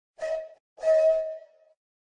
Ideal Oliver whistle
This is what Oliver the Great Western Engine's whistle sounds like but in reality.

Created by Audacity.